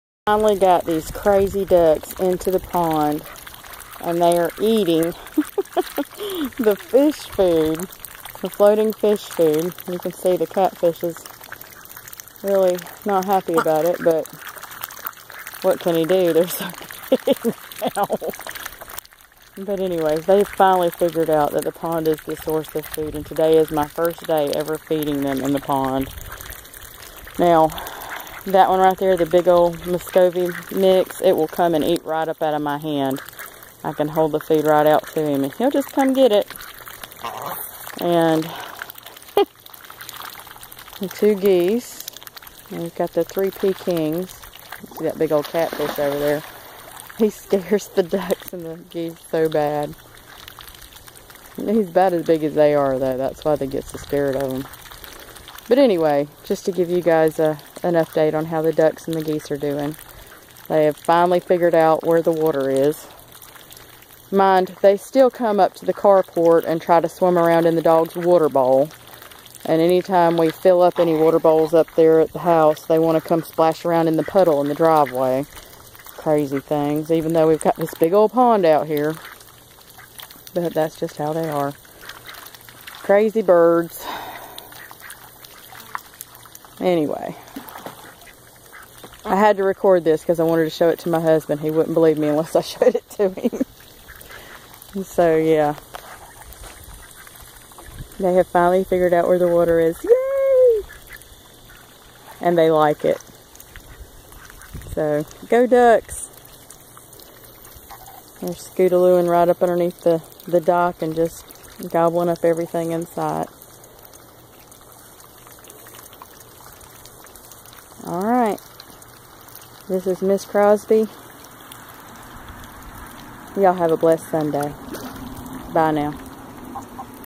Our ducks and geese swim!
Our ducks and geese are finally swimming in the pond.